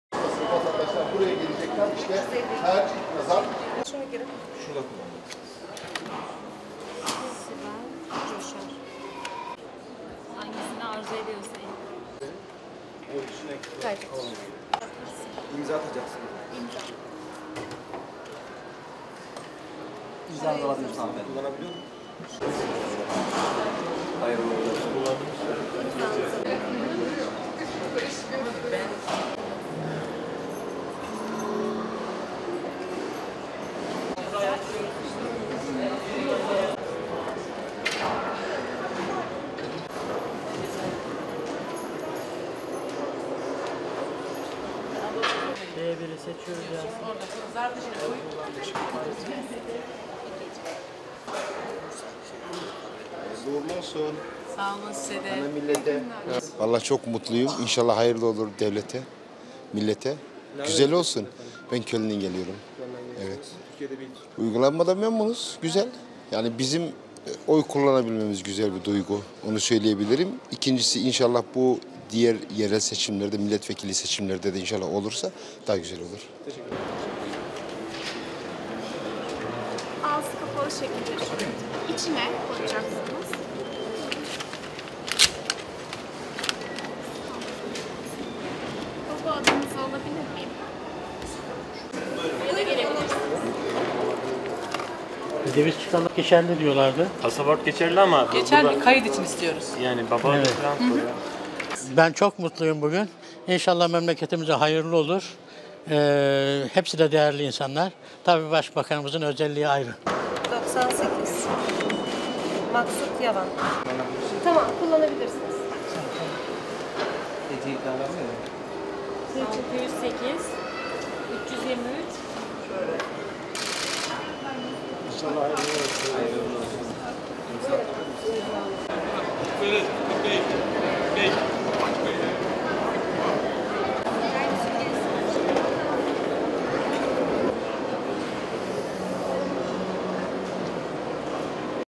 Cumhurbaşkanlığı seçimleri için ilk oylar kullanılmaya başladı
10 Ağustos’ta yapılacak Cumhurbaşkanlığı seçimleri için gümrük kapılarında oy verme işlemleri bugün itibariyle başladı.